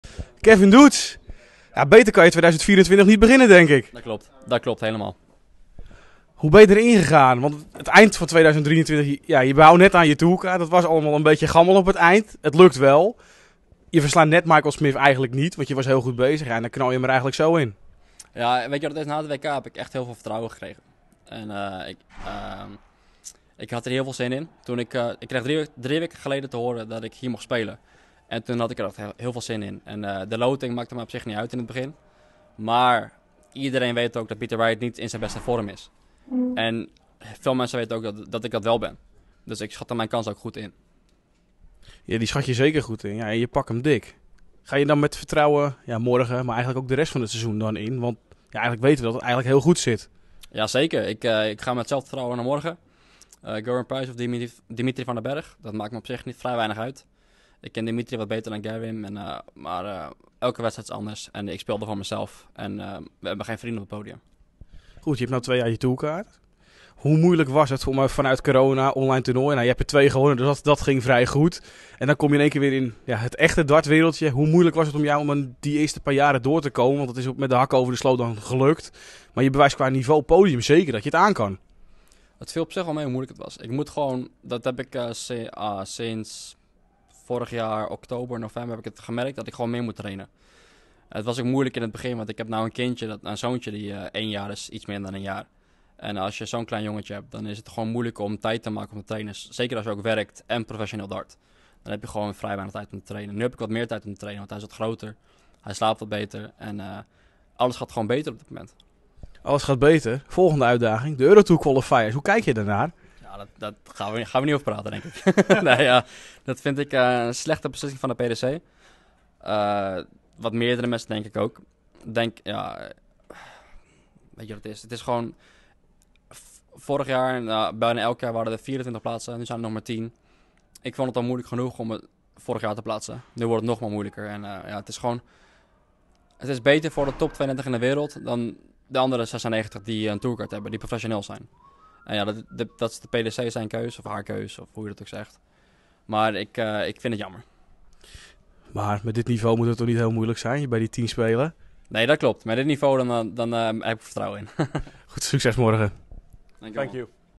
Kevin Doets: 'Na het WK heb ik heel veel vertrouwen gekregen' | TOTO Dutch Darts Masters 2024
In het weekend van 26 en 27 januari wordt in de Maaspoort in Den Bosch de TOTO Dutch Darts Masters georganiseerd, een toernooi dat deel is van de World Series of Darts. Op de openingsavond mochten acht PDC-topspelers, waaronder Michael van Gerwen, het opnemen tegen 7 Nederlanders en één Belg.

In de tweede partij van de avond wist Kevin Doets met 6-2 te winnen van Peter Wright. Na afloop sprak Darts Actueel met ‘Hawkeye’.

Al het dartnieuws waaronder de Dutch Darts Masters is terug te vinden op onze website!